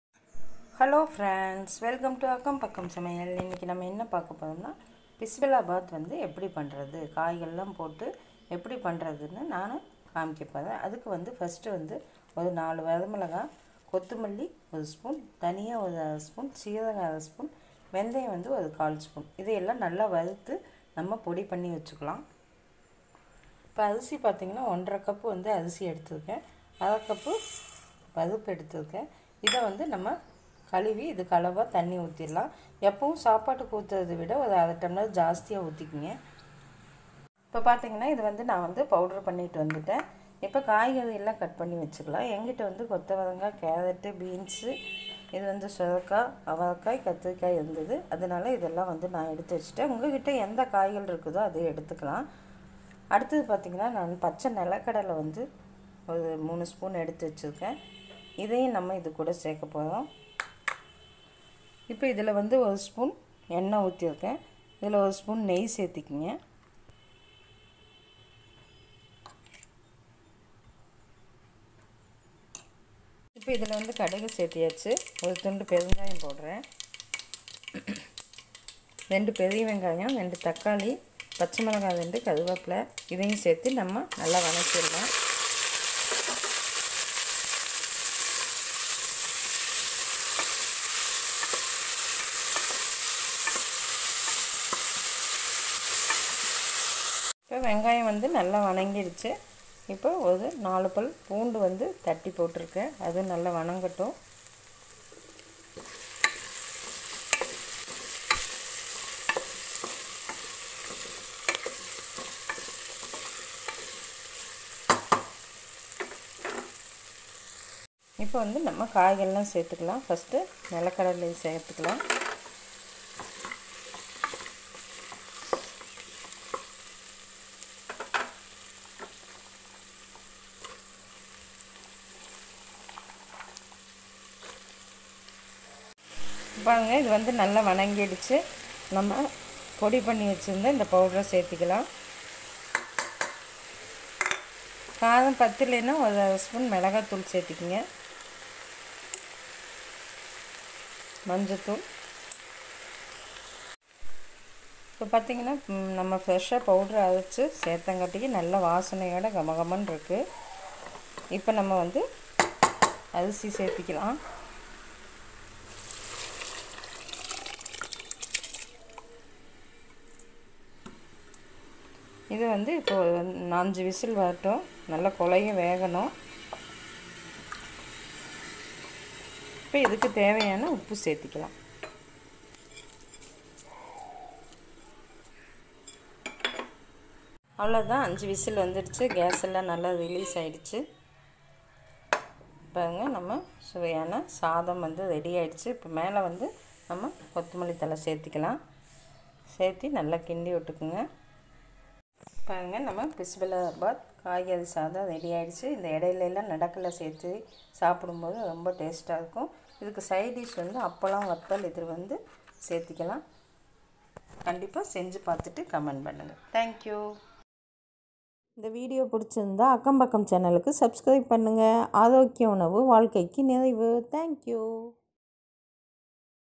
cooker#how to make Bisibelabath||in Tamil பிஸிபெலாபாத் சாம்பார் சாதம்135
Mixed vegetables 3/4kg
(Kothaavarakai,beans,carrot, Suraikai,Avarakai,brinjal)
Raw Ground nuts-3spn
Rice -1/1/2cup(enyrice)
Door Dall -1/2 cup
Masala Powder:👉
Red Chilly -4
Dhani
ya-1spn
Pepper seed-1spn
Seeragam-1spn
Vendayam -1/2spn
Onion-2
Tomata-2
Green Chilly-2
Coriander leaves- needed
Red Chilly powder 1 spn